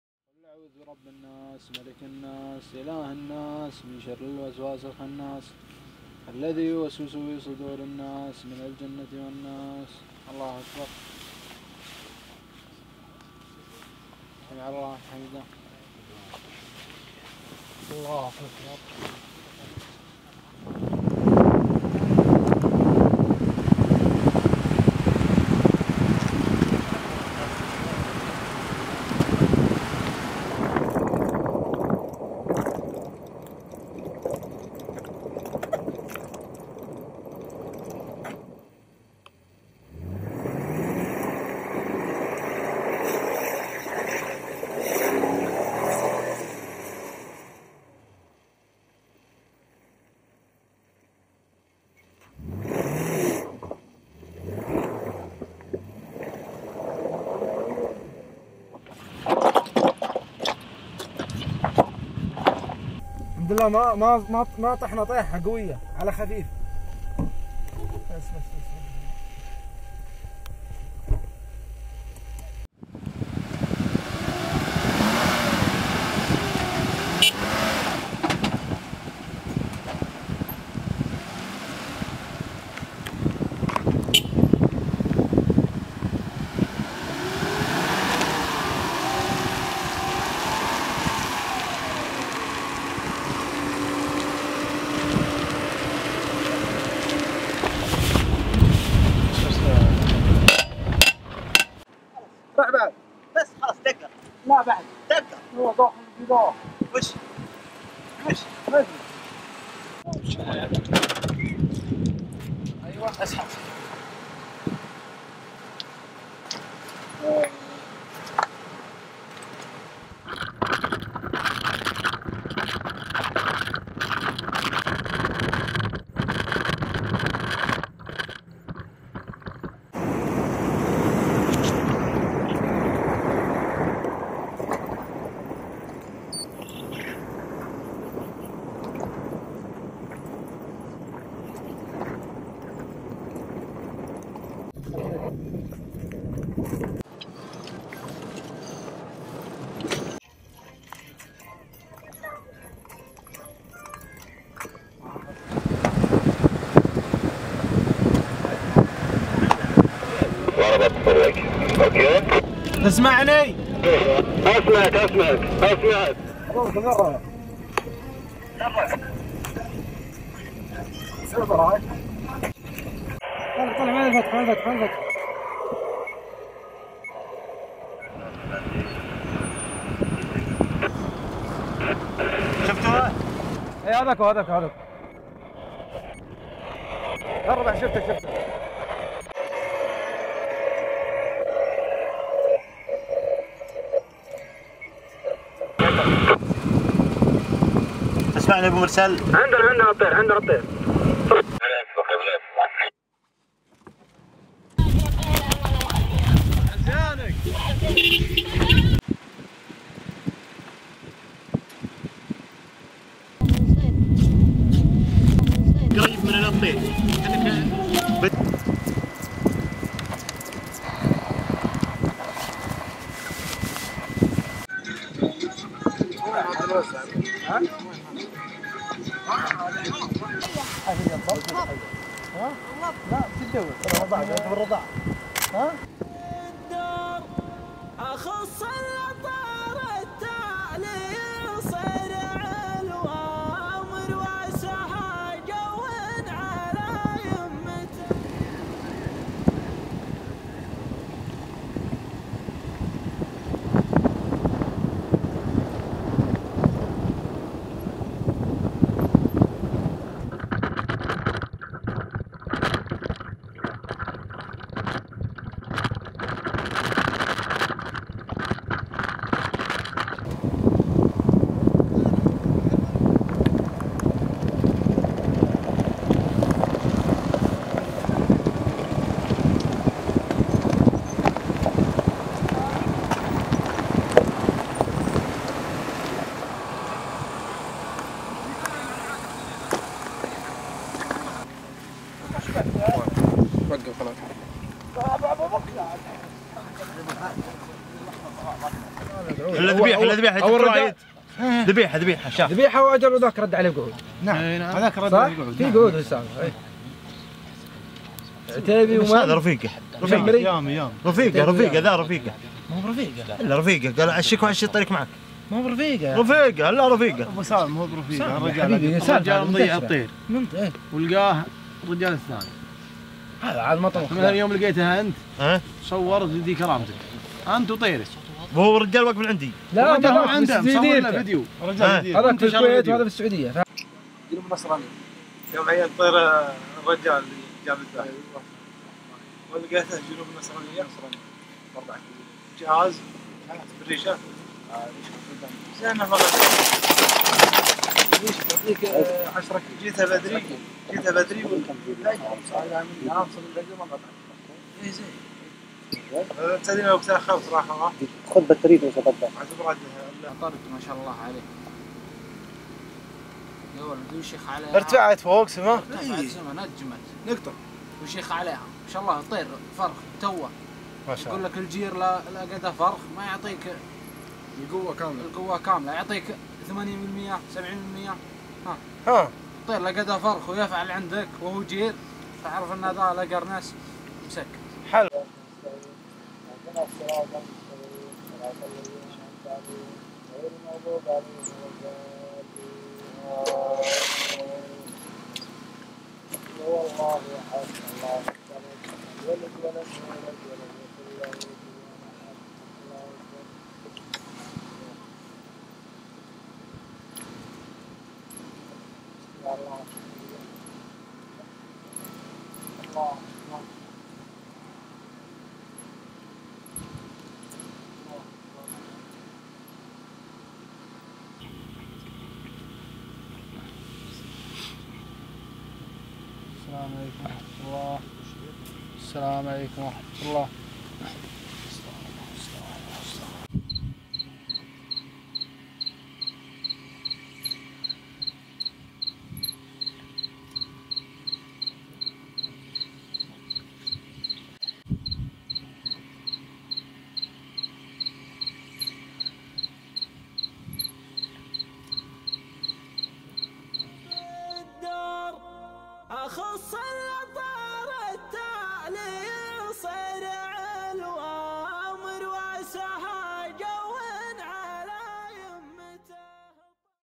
The Falconer
The Story of a Relationship, Loyalty and Trust
Two falconers Abdualh and Saoud invited me to their tent in the middle of the desert and took me for a hunting day full of excitement and surprises.